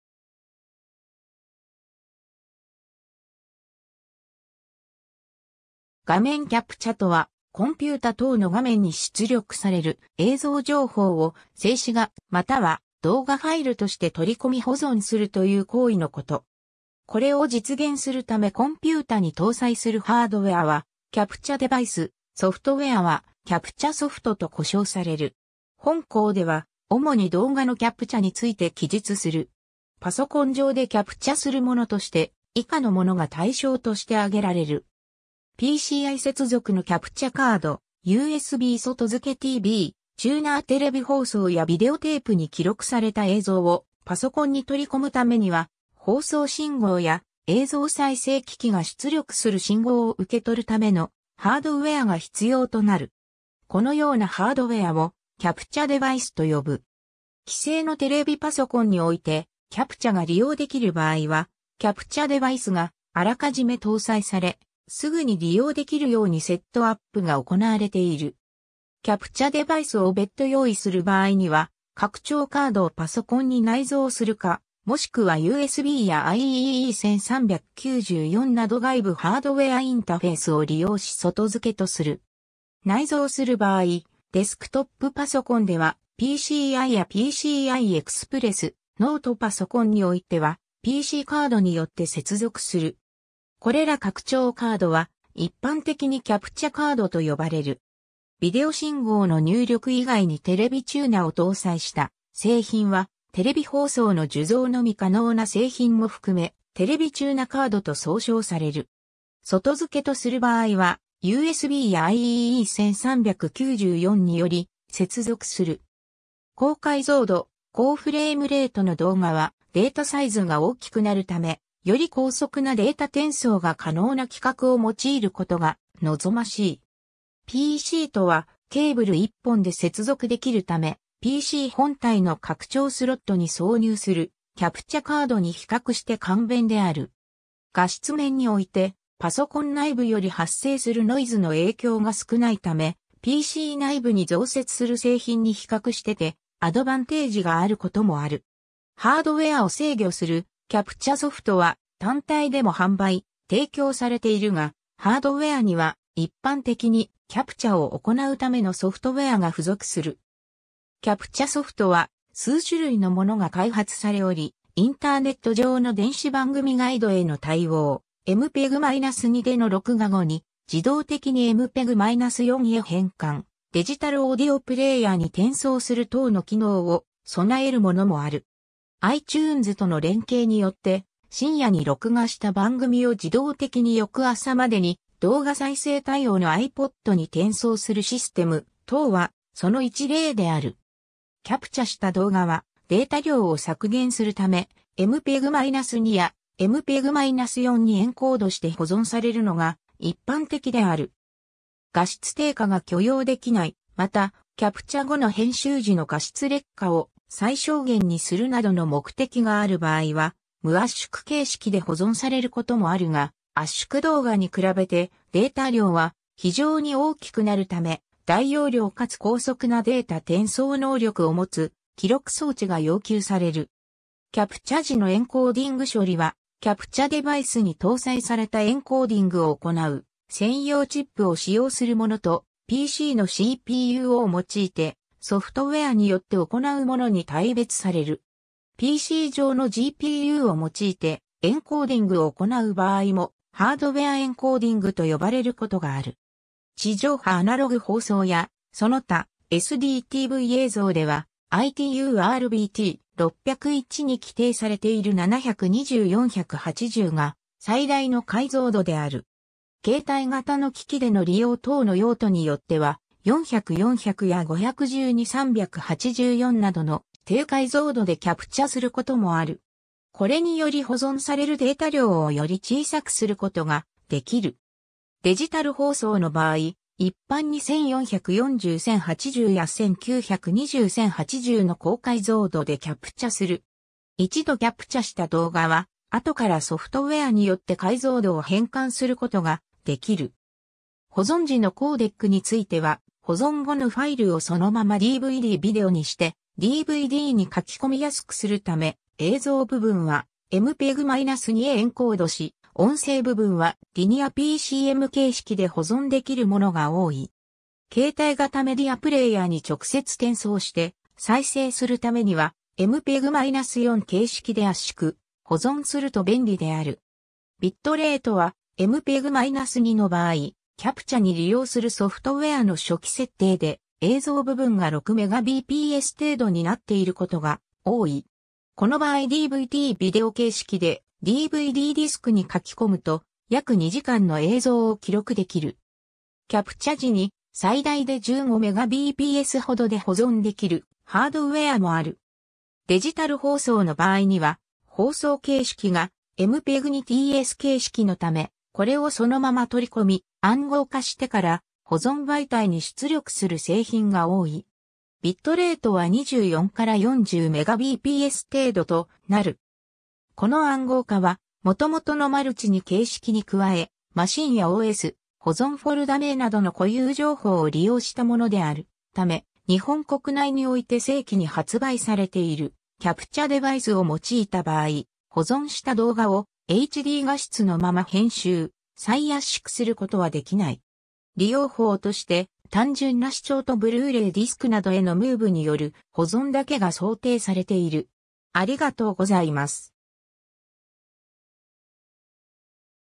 キャプチャ (録画ソフト)
画面キャプチャ (video capture) とは、コンピュータ等の画面に出力される映像情報を静止画または動画ファイルとして取り込み保存するという行為のこと。
これを実現するためコンピュータに搭載するハードウェアは「キャプチャデバイス」、ソフトウェアは「キャプチャソフト」と呼称される。
本稿では、主に動画のキャプチャについて記述する。
 パソコン上でキャプチャするものとして、以下のものが対象として挙げられる。
 PCI接続のキャプチャカード USB外付けTVチューナー テレビ放送やビデオテープに記録された映像をパソコンに取り込むためには、放送信号や、映像再生機器が出力する信号を受け取るためのハードウェアが必要となる。
このようなハードウェアをキャプチャデバイスと呼ぶ。
既製のテレビパソコンにおいて、キャプチャが利用できる場合は、キャプチャデバイスが予め搭載され、すぐに利用できるようにセットアップが行われている。
 キャプチャデバイスを別途用意する場合には、拡張カードをパソコンに内蔵するか、もしくはUSBやIEEE 1394など外部ハードウェアインタフェースを利用し外付けとする。
内蔵する場合、デスクトップパソコンではPCIやPCI Express、ノートパソコンにおいてはPCカードによって接続する。
これら拡張カードは一般的にキャプチャカードと呼ばれる。
ビデオ信号の入力以外にテレビチューナを搭載した製品は、テレビ放送の受像のみ可能な製品も含めテレビチューナカードと総称される。
 外付けとする場合は、USBやIEEE 1394により接続する。
高解像度・高フレームレートの動画はデータサイズが大きくなるため、より高速なデータ転送が可能な規格を用いることが望ましい。
 PCとはケーブル一本で接続できるため、PC本体の拡張スロットに挿入するキャプチャカードに比較して簡便である。
画質面においてパソコン内部より発生するノイズの影響が少ないためPC内部に増設する製品に比較しててアドバンテージがあることもある。
 ハードウェアを制御するキャプチャソフトは単体でも販売・提供されているが、ハードウェアには一般的にキャプチャを行うためのソフトウェアが付属する。
 キャプチャソフトは数種類のものが開発されおり、インターネット上の電子番組ガイド (iEPG) への対応、MPEG-2での録画後に自動的にMPEG-4へ変換、デジタルオーディオプレーヤーに転送する等の機能を備えるものもある。
 iTunesとの連携によって、深夜に録画した番組を自動的に翌朝までに動画再生対応のiPodに転送するシステム等はその一例である。
 キャプチャした動画はデータ量を削減するため、MPEG-2やMPEG-4にエンコードして保存されるのが一般的である。
画質低下が許容できない、また、キャプチャ後の編集時の画質劣化を最小限にするなどの目的がある場合は無圧縮形式で保存されることもあるが、圧縮動画に比べてデータ量は非常に大きくなるため、大容量かつ高速なデータ転送能力を持つ記録装置が要求される。
 キャプチャ時のエンコーディング処理は、キャプチャデバイスに搭載されたエンコーディングを行う専用チップを使用するものと、PCのCPUを用いてソフトウェアによって行うものに大別される。
PC上のGPUを用いてエンコーディングを行う場合も、ハードウェアエンコーディングと呼ばれることがある。
 地上波アナログ放送やその他SDTV映像では、ITU-R BT.601に規定されている720×480 (NTSCの場合)が最大の解像度である。
携帯型の機器での利用等の用途によっては、400×400や512×384などの低解像度でキャプチャすることもある。
これにより保存されるデータ量をより小さくすることができる。
デジタル放送の場合、一般に1440×1080（地上波デジタル）や1920×1080（BS/CS110）の高解像度でキャプチャする。
一度キャプチャした動画は、後からソフトウェアによって解像度を変換することができる。
 保存時のコーデックについては、保存後のファイルをそのままDVD-VideoにしてDVDに書き込みやすくするため、映像部分はMPEG-2 へエンコードし、音声部分はリニアPCM形式で保存できるものが多い。
携帯型メディアプレーヤーに直接転送して再生するためにはMPEG-4形式で圧縮、保存すると便利である。
 ビットレートはMPE...